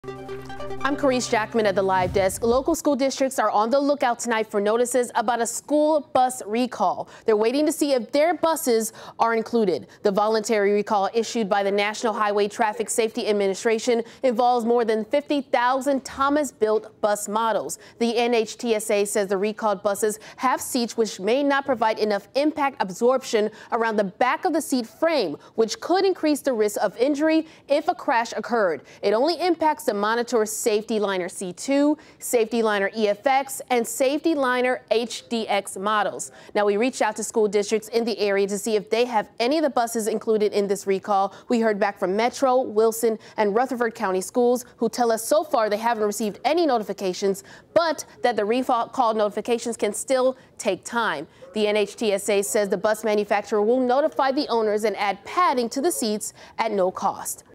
School Bus Recall
Thomas-Built buses are being recalled